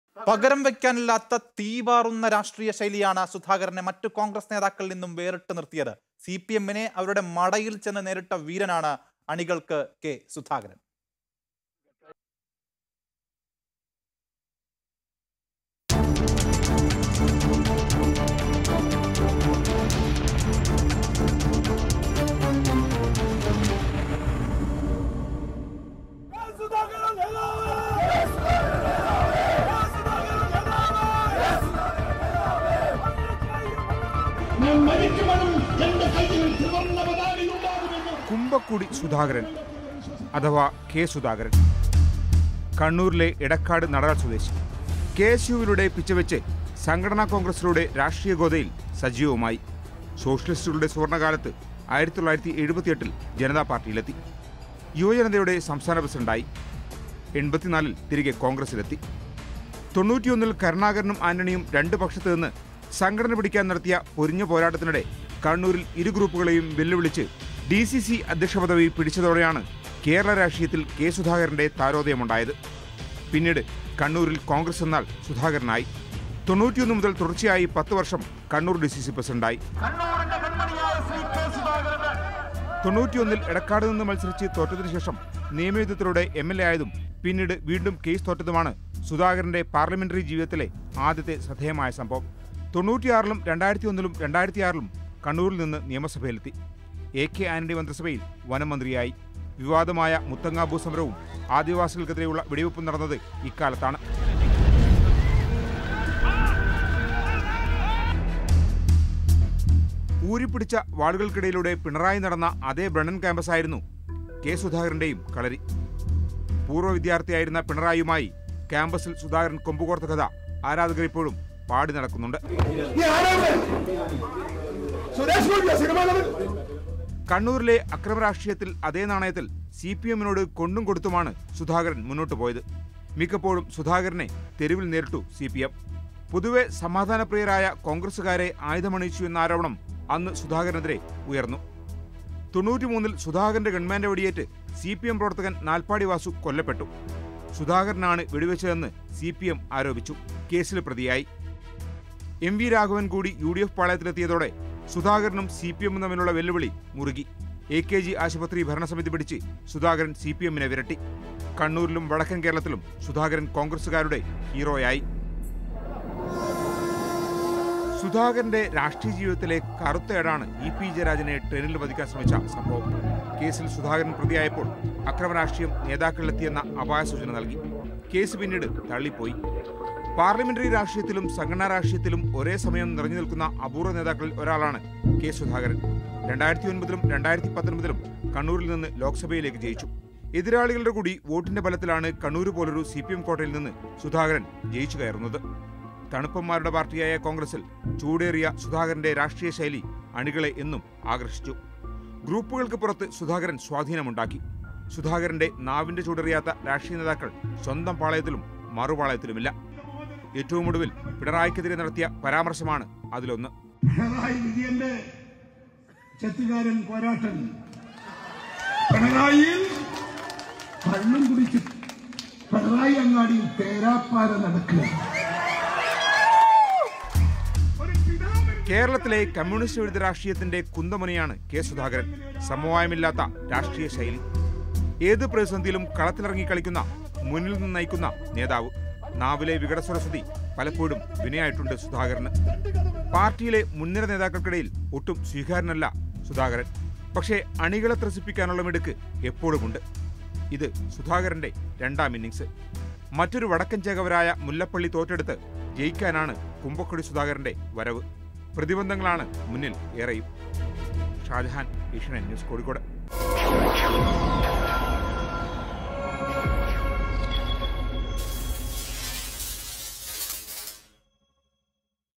അനുയായിയില്‍ നിന്ന് അമരത്തേക്ക്; കോണ്‍ഗ്രസില്‍ ഇനി സുധാകരന്‍ യുഗം K SUDHAKARAN : Bio, Political life
അനുയായിയില്‍ നിന്ന് അമരത്തേക്ക്, പകരം വെയ്ക്കാനില്ലാത്ത തീപാറുന്ന രാഷ്ട്രീയ ശൈലി;
സിപിഎമ്മിനെ അവരുടെ മടയില്‍ ചെന്ന് നേരിട്ട വീരനാണ് അണികള്‍ക്ക് കെ.സുധാകരന്
Facts About K Sudhakaran: Know K Sudhakaran's plotical background and political life